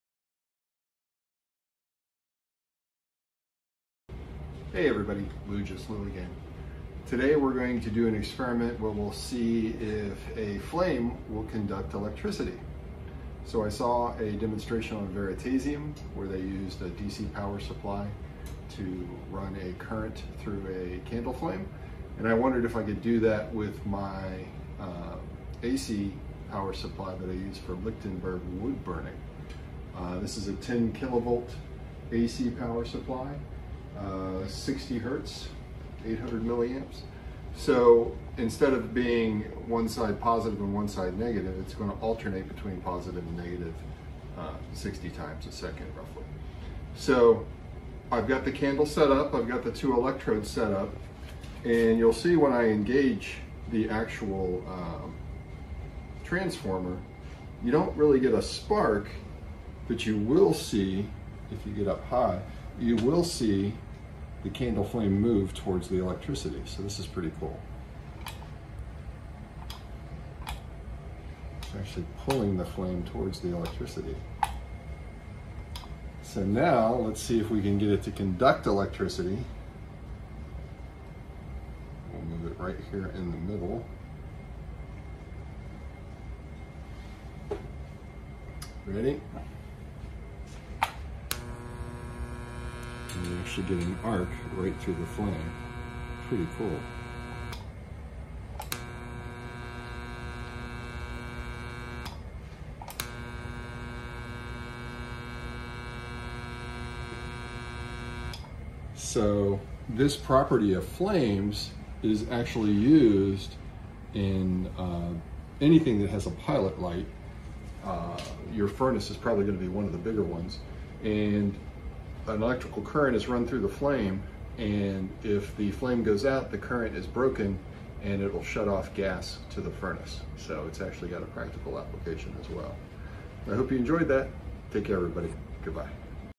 Conduct Electricity in a Flame
Will a flame serve as a conductor of electricity? Let's find out!

This video is a slightly different approach to the one from Veritasium that sparked the idea. :)